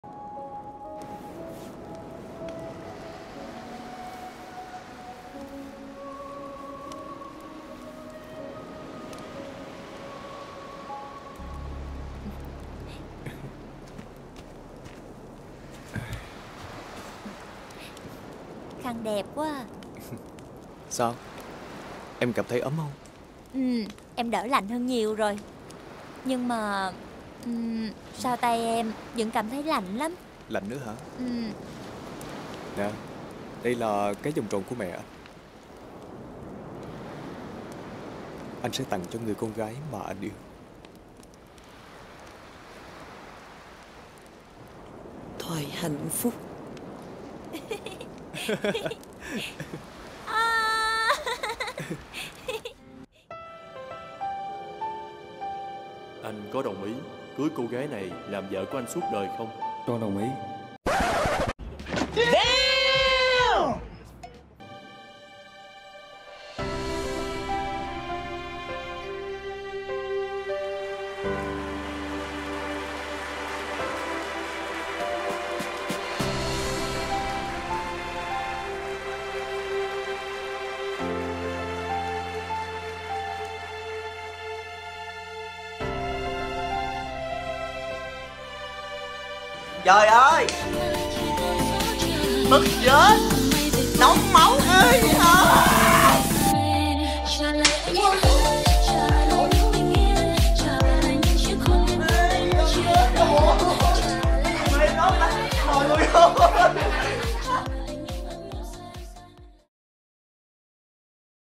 Thanh niên chung tình cầu hôn “não cá vàng” - KC 04.06.2020
Bộ phim xoay quanh cuộc sống của một nhà thiết kế thời trang Huyền (Khánh Hiền). Mặc dù rất tài năng, nhưng nàng ta lại mắc phải căn bệnh Alzheimer (sa sút trí tuệ), khiến bản thân luôn quên trước quên sau. Do chẳng thể chịu nổi cái tính nhớ nhớ quên quên ở nàng ta, anh chồng tương lai (Tuấn Trần) đã thẳng thắn đề nghị từ hôn. Đau khổ và suy sụp, cô tình cờ gặp gỡ chàng nhiếp ảnh gia có hành tung bí ẩn (La Thành). Kì lạ thay, dẫu biết rõ bệnh tình của đối phương, tuy nhiên, anh ta vẫn quyết định tỏ tình, thậm chí tuyên bố sẵn sàng lấy nhà thiết kế làm vợ